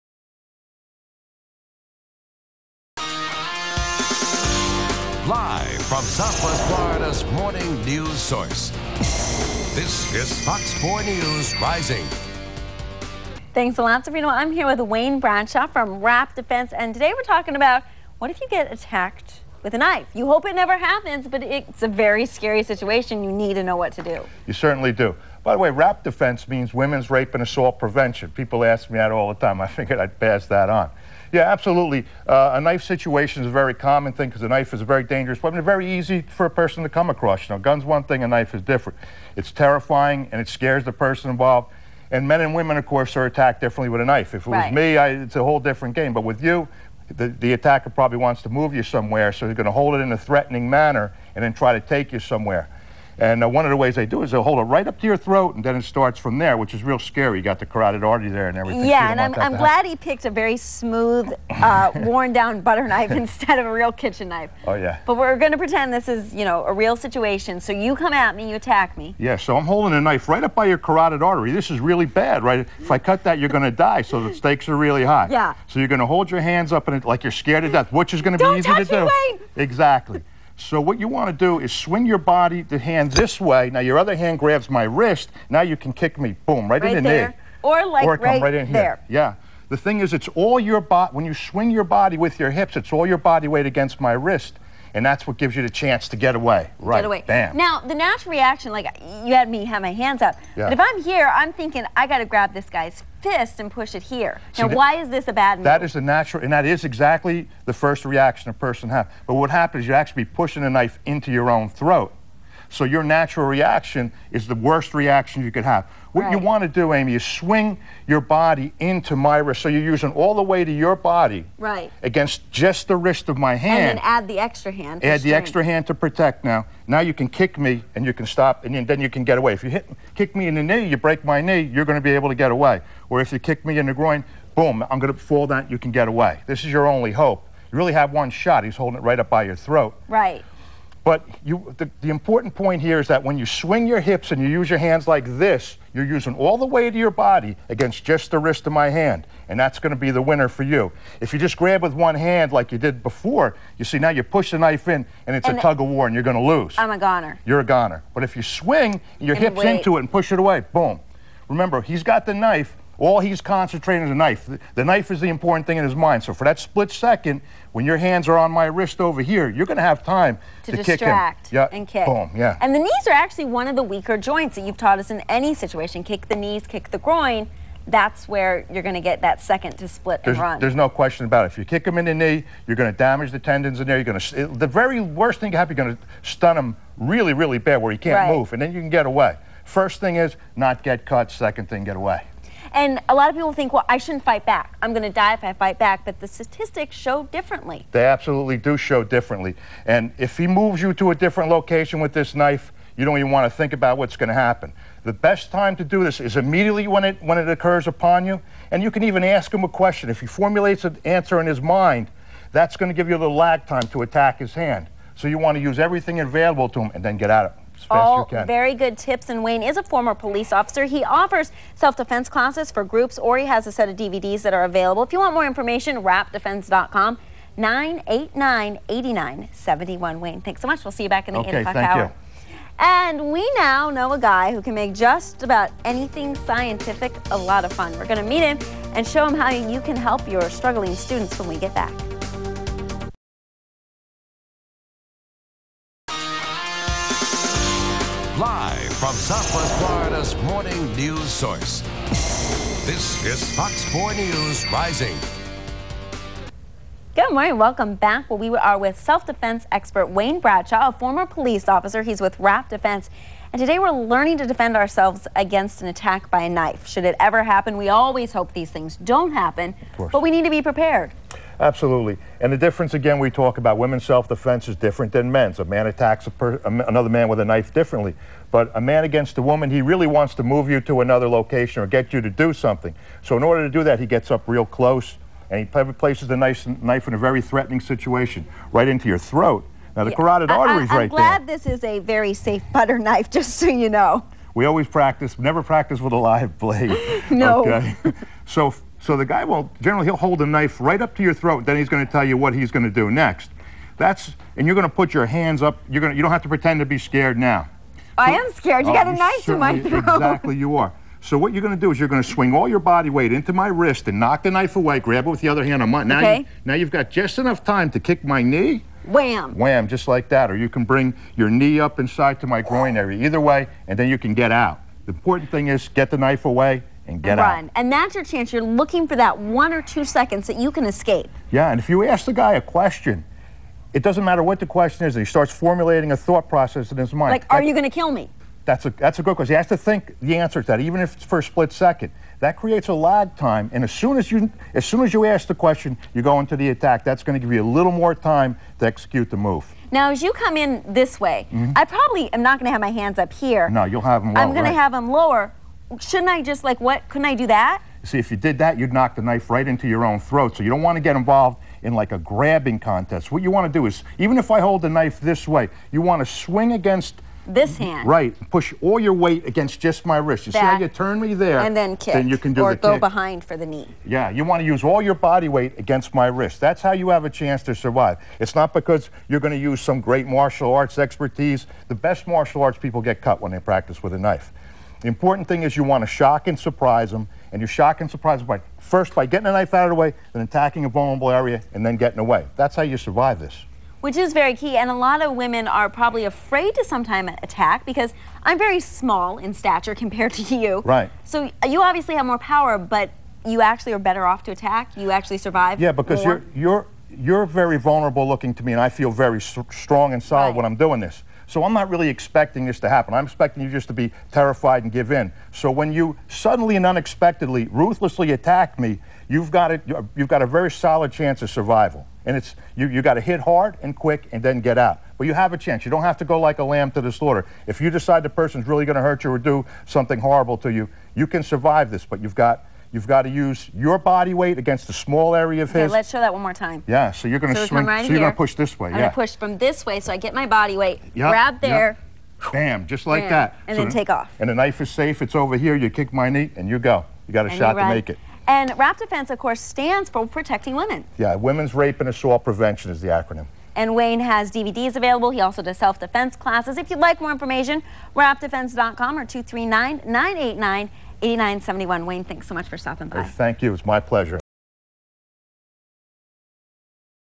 Self Defense For Women - WRAP Defense -Wayne Bradshaw on Fox 15
Wayne Bradshaw teaching his revolutionary WRAP Defense.  Check out WRAPDefense.com